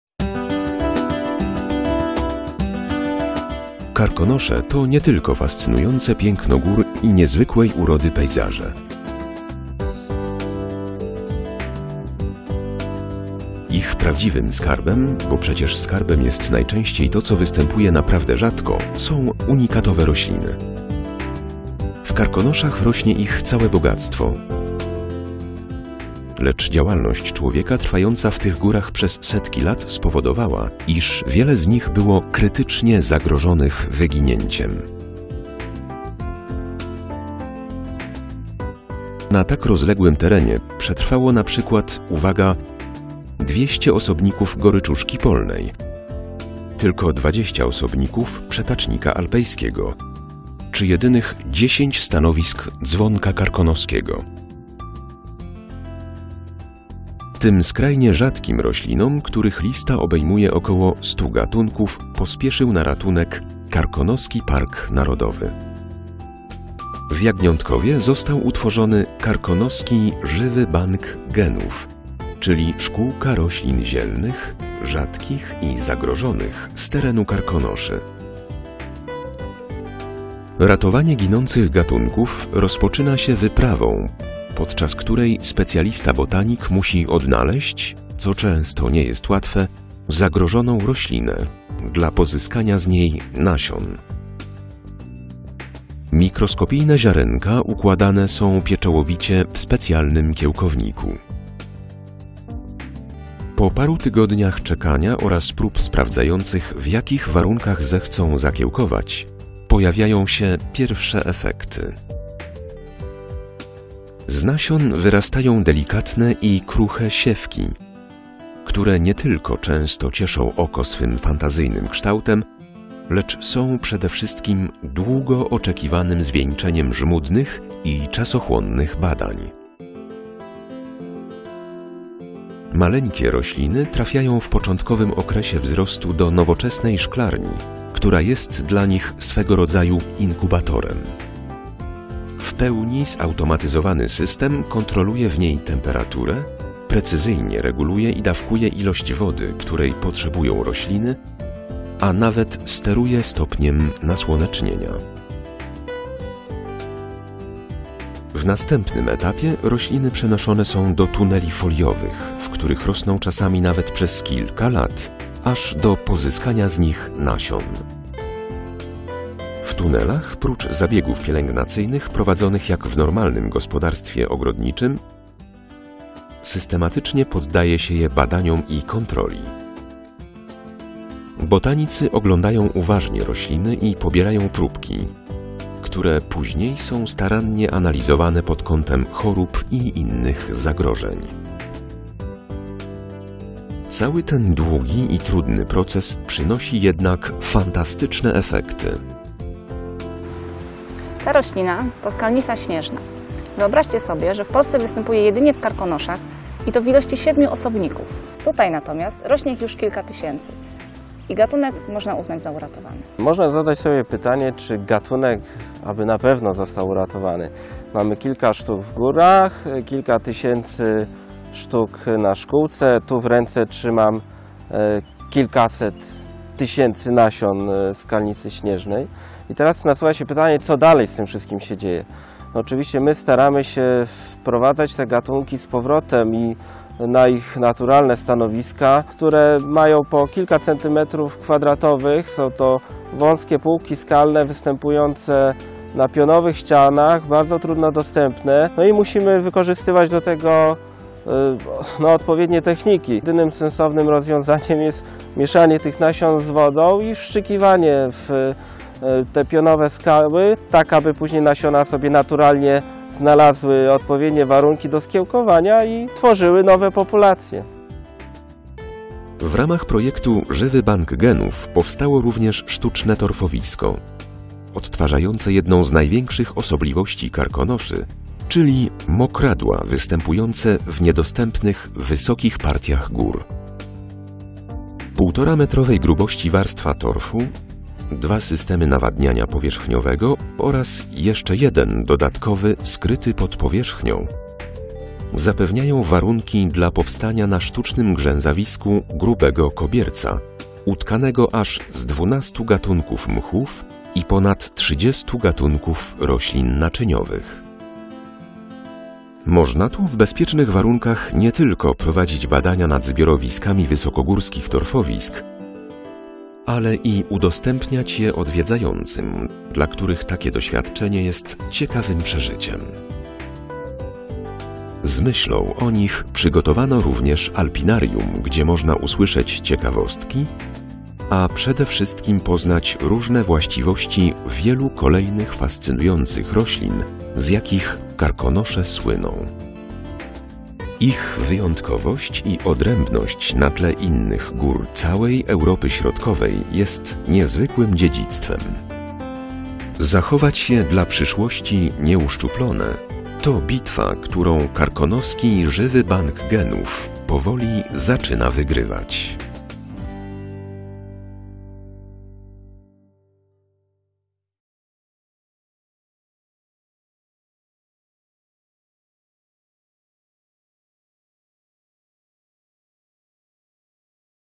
Żywy Bank Genów Karkonoskiego Parku Narodowego
Żywy Bank Genów Karkonoskiego Parku Narodowego, to miejsce w którym specjaliści ratują cenne gatunki roślin. Aż trudno uwierzyć, że w Karkonoszach są rośliny występujące w ledwie kilku egzemplarzach.